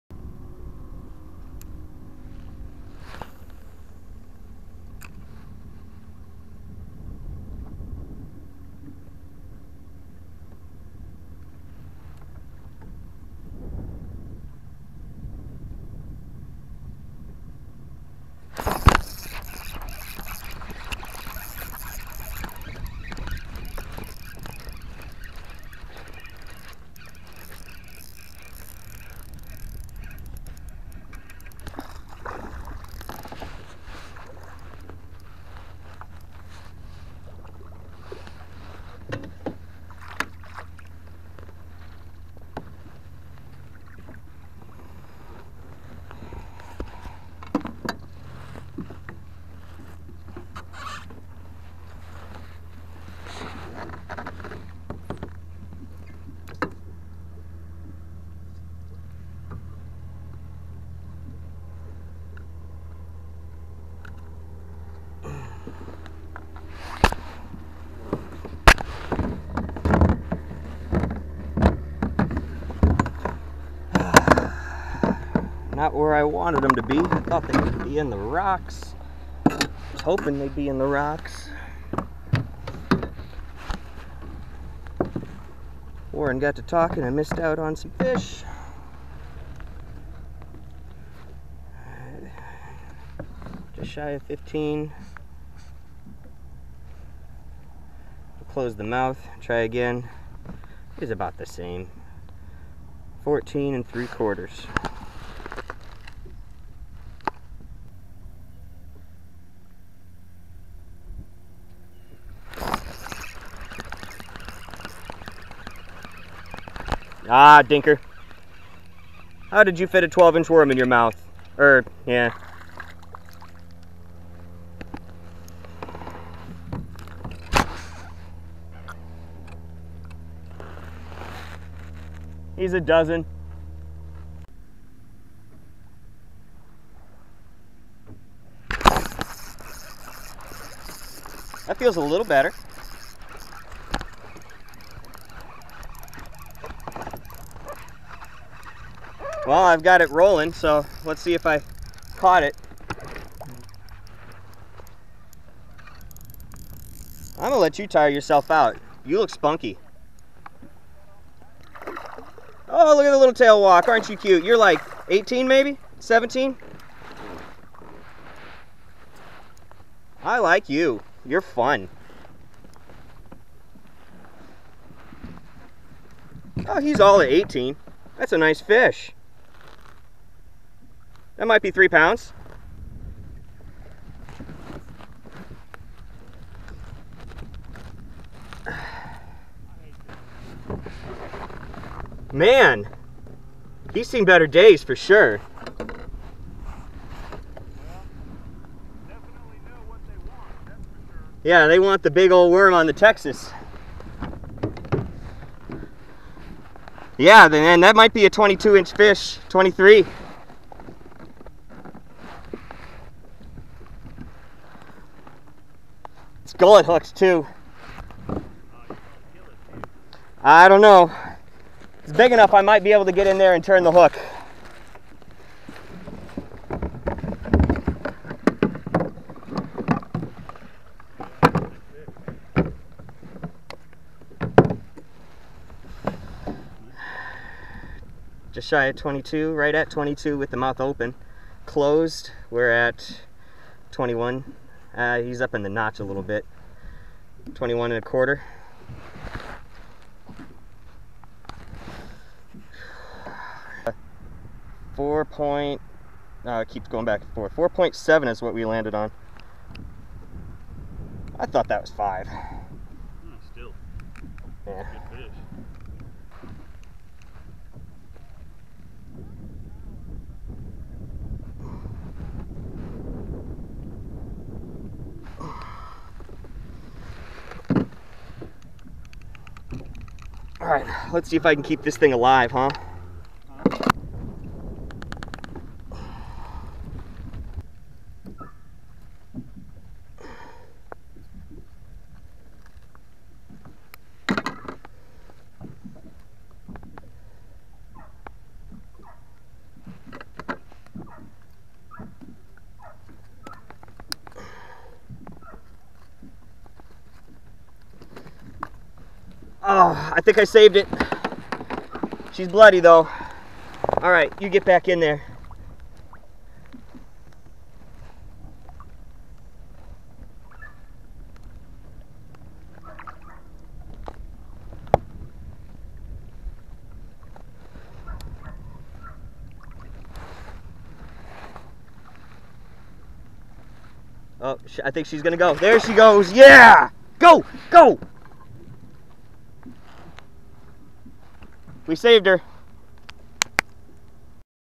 9-16-18 Lavon - Lavonia Park
Kayak ass fishing Lake Lavon. Tried whopper ploppers and 3 different crank bait depths along the dam, then moved north with a Texas rigged 10" power worm and found a pattern. I was hoping they would be transitioning to a good fall crank bite, but they're not there yet.

The big girl at the end was super beat up! I have no idea how she got so beat and bloody. She also had a busted gill. Now she also has a hole in her gullet, but at least she's still swimming! I forgot to hit record before the cast, so the super-fun fight to get her to the boat is missing :(